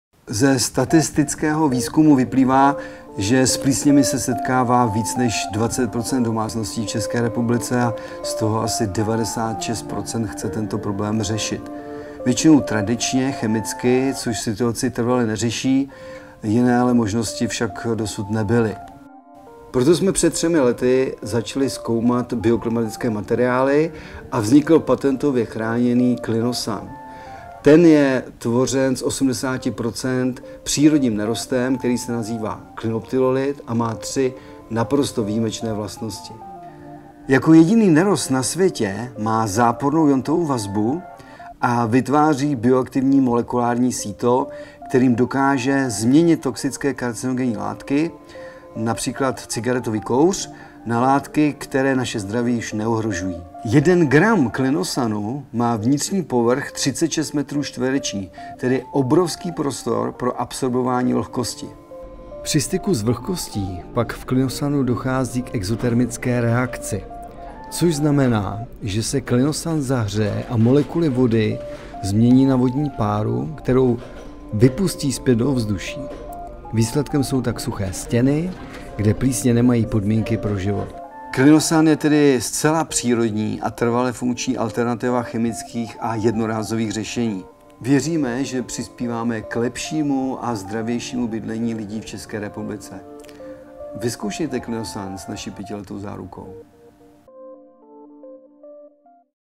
Realsan - Klinosan, trvalé řešení plísní bez chemie
Klinosan je stěrka na bázi klinoptilolitu (druh nerostu zeolit), která zajistí trvalé vyřešení plísní v interiéru. Pokud tedy také každý měsíc stříkáte chemické spreje do rohů v koupelně, na chodbě, v kuchyni a na dalších místech a už byste s tím chtěli nadobro skončit, je Klinosan přesně pro vás.